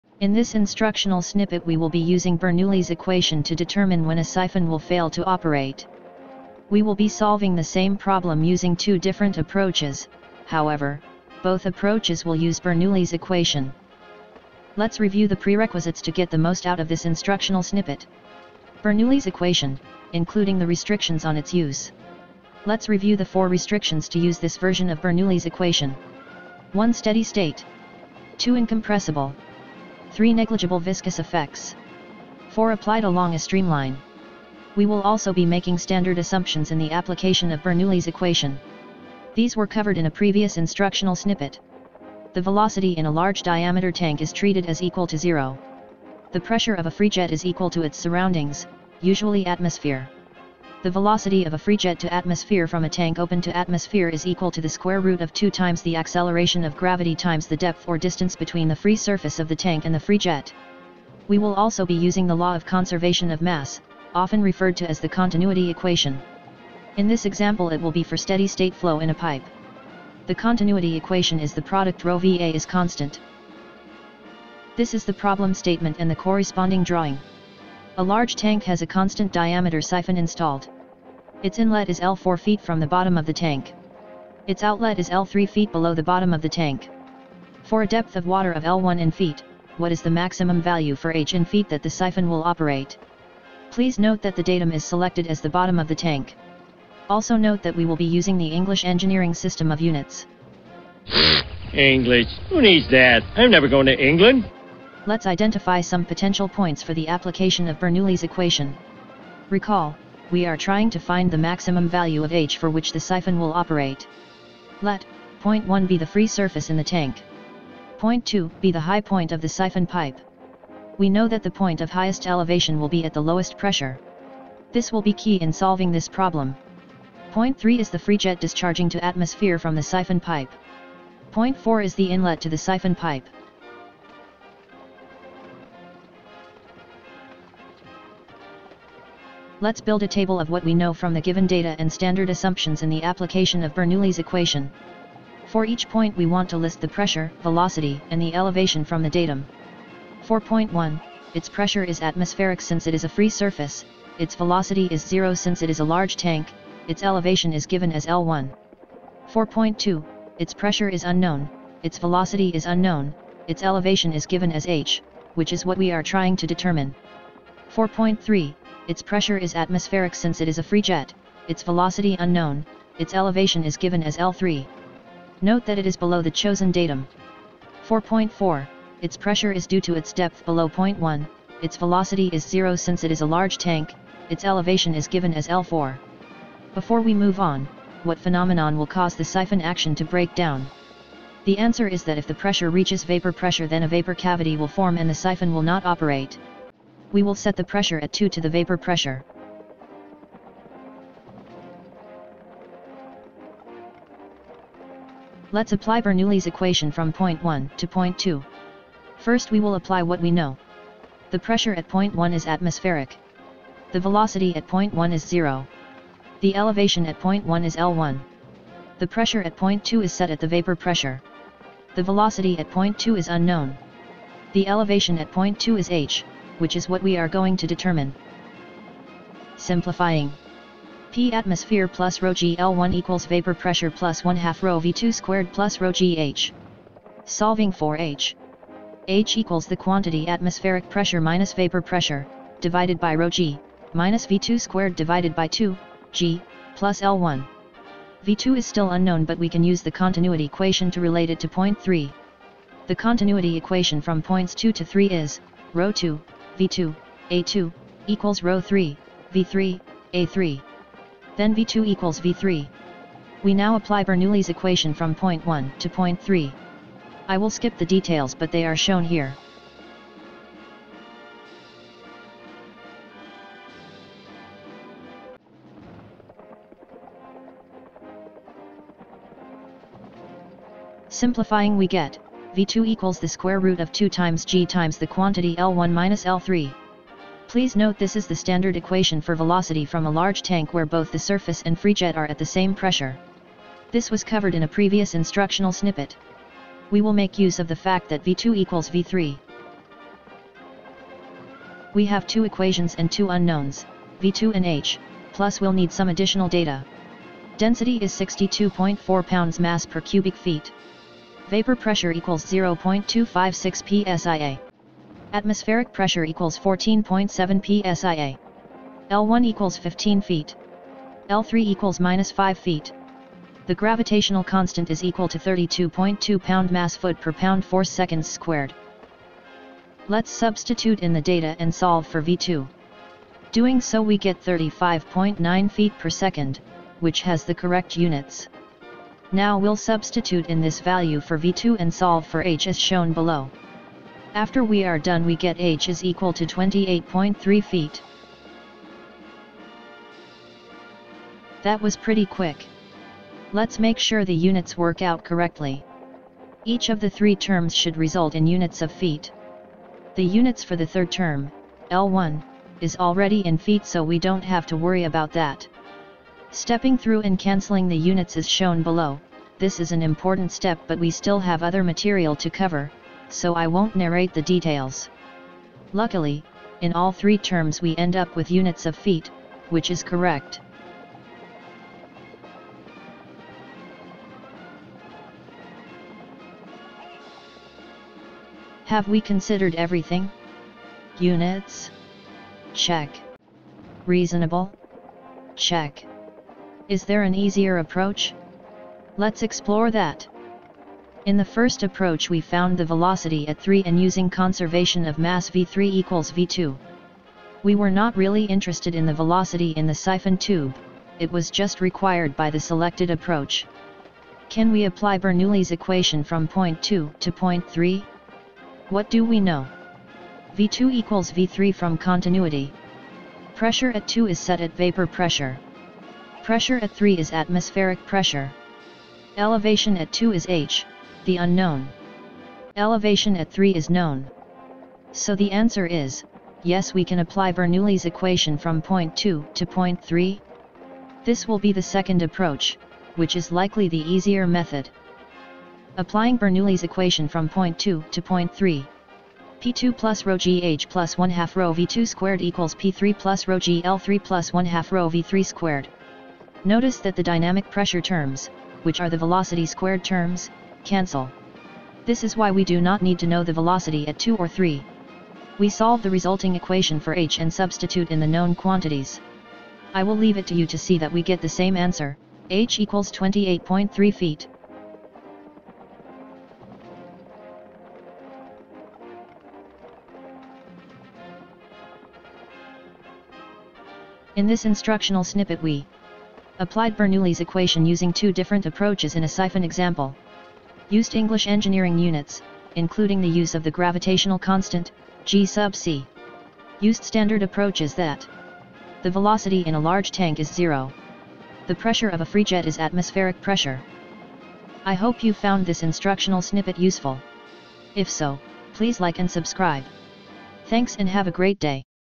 Bernoulli's Equation Example
Bernoulli'sEquation, Bernoulli, Bernoullis, Siphon, Fluid Mechanics, Bernoulli example, Engineering, MechanicalEngineering. This example uses Bernoulli's equation to analyze the maximum elevation for a siphon to operate.